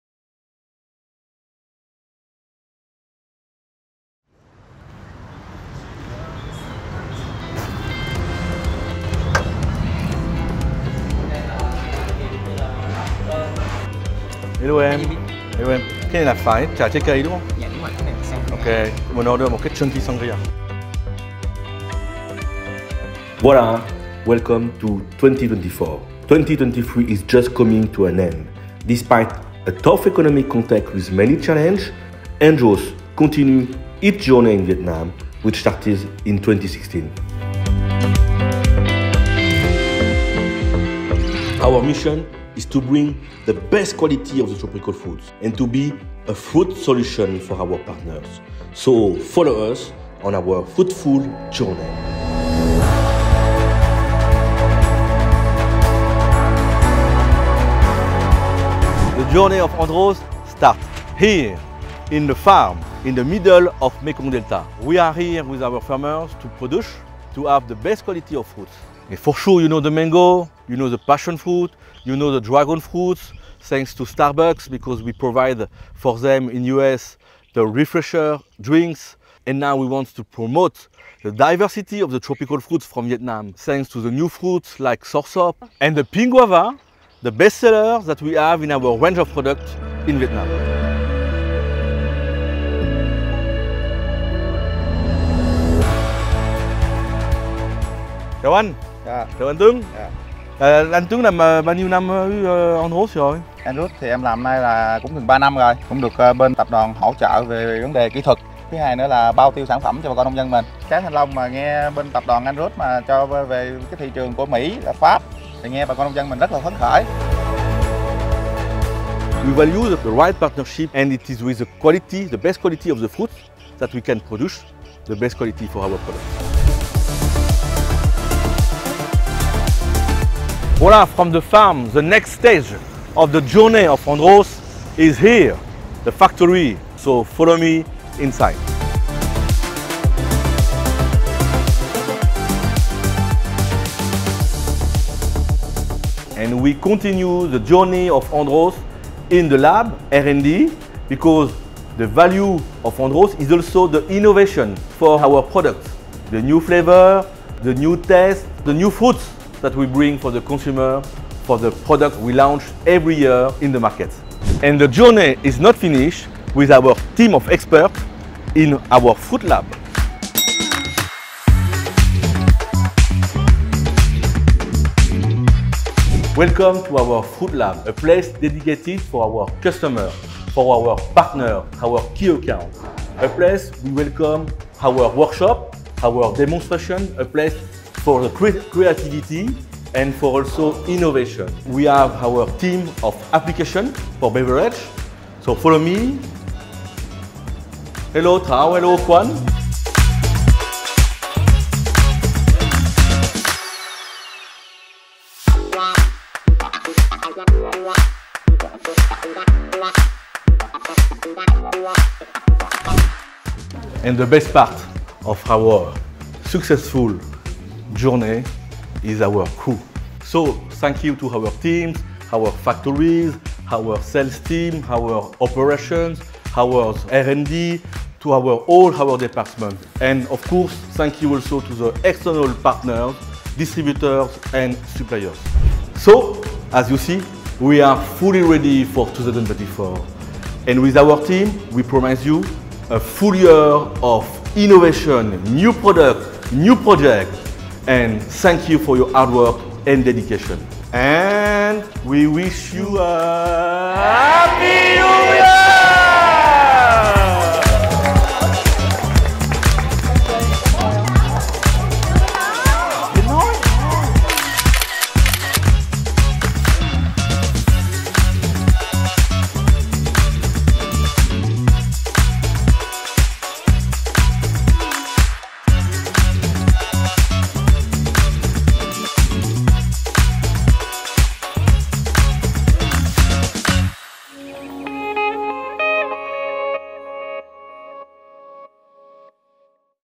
Chào mừng 2024 - Welcome to 2024 | Andros Asia (Engsub)
Greetings in 2024! Andros remains steadfast in its commitment to advancing in Vietnam, aspiring to be the ultimate "fruit solution" for our valued partners while pioneering innovation. As we navigate the midpoint between the start of the New Year and the upcoming Lunar New Year, we want to extend our sincere gratitude to our dedicated team for their relentless efforts, unwavering commitment and hard work throughout 2023.

We want to express our best wishes for a prosperous and successful 2024 to our valued partners, suppliers, and customers. Let us persist in our collective efforts, ensuring a year of accomplishments with a dedicated focus on Corporate Social Responsibility (CSR).

Andros Asia đã sẵn sàng chào đón năm 2024 thật bùng nổ! Bắt đầu từ 2016, Andros tiếp tục "hành trình quả ngọt" tại Việt Nam, cam kết không ngừng phát triển để mang lại những "giải pháp trái cây" tối ưu cho các đối tác, khách hàng và đồng thời bước sang hành trình mới với nhiều sự sáng tạo đổi mới. Trong khi chúng ta đi qua giữa đường vào Năm Mới và Tết Nguyên Đán sắp tới, chúng tôi muốn dành khoảnh khắc này để bày tỏ lòng biết ơn chân thành đến đội ngũ đầy cam kết của chúng tôi vì những nỗ lực không ngừng, cam kết vững vàng và làm việc chăm chỉ suốt năm 2023.

Chúng tôi mong muốn gửi lời chúc tốt đẹp nhất cho một năm mới phồn thịnh và thành công đến các quý khách hàng, đối tác và toàn thể đội ngũ Andros Asia. Hãy kiên trì trong những nỗ lực chung của chúng ta, đảm bảo một năm đầy thành tựu, với sự tập trung đặc biệt vào Trách nhiệm Xã hội Doanh nghiệp (CSR).